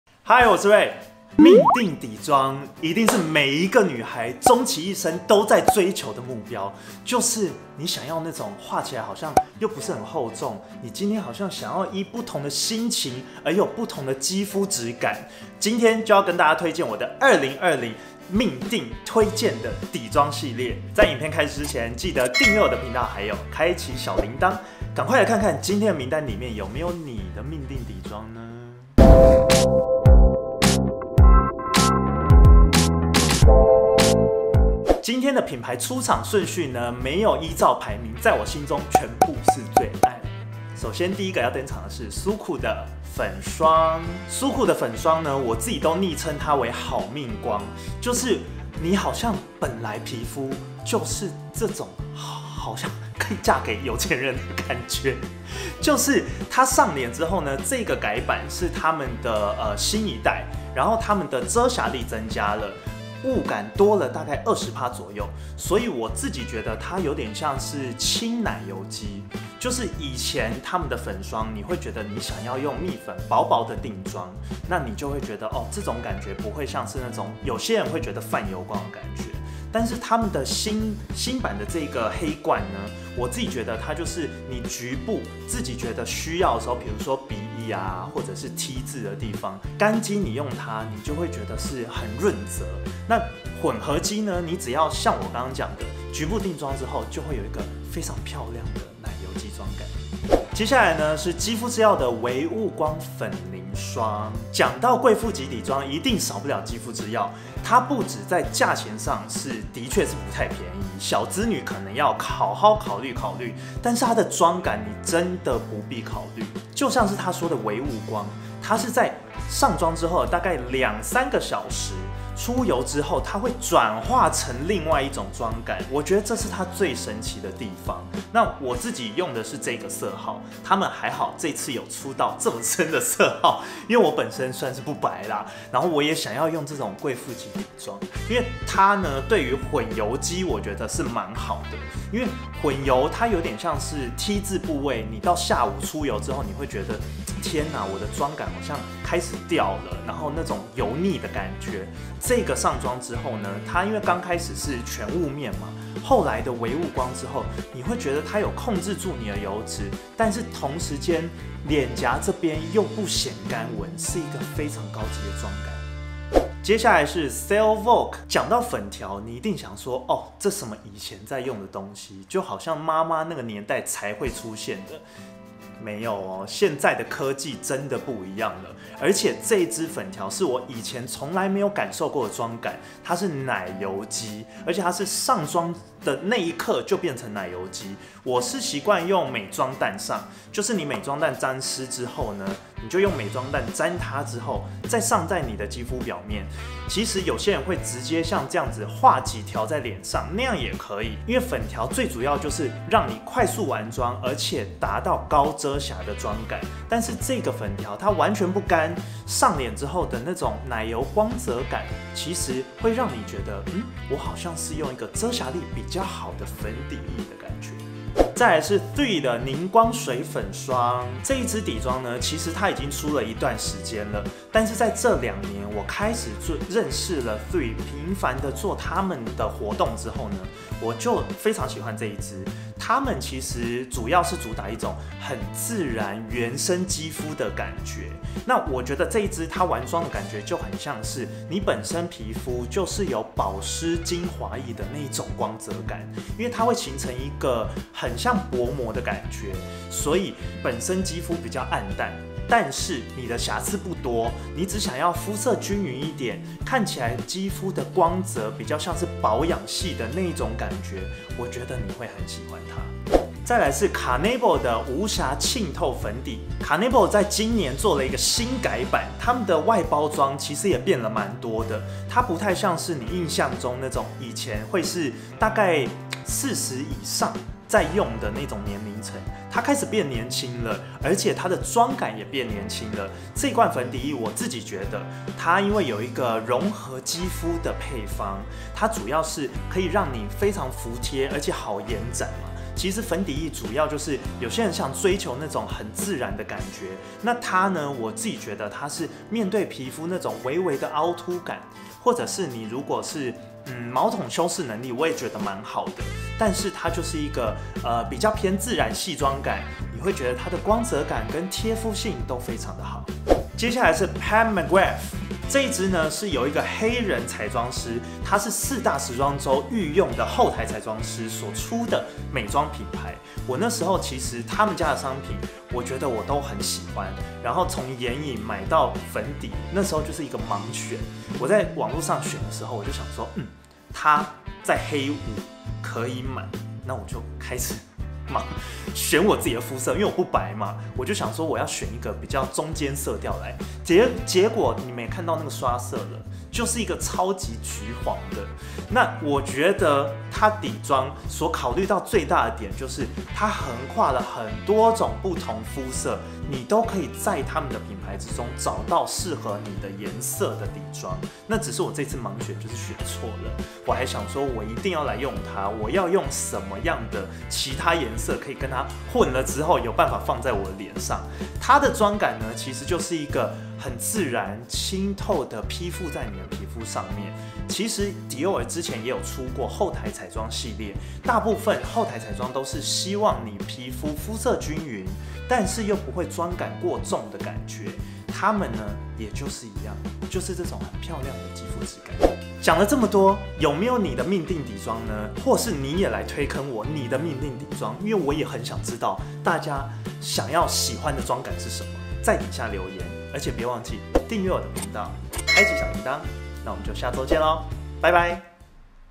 年度底妝爭霸戰 | 年後帶著你的命定底妝回歸職場！
奶油光！半霧光！到底要選誰？
年度最愛粉底液看2021還會不會在我的名單上，平常化底妝的時間那麼長，當然要選讓皮膚看起來漂亮的那一個！
品牌的喜愛度不分先後順序，都很愛😍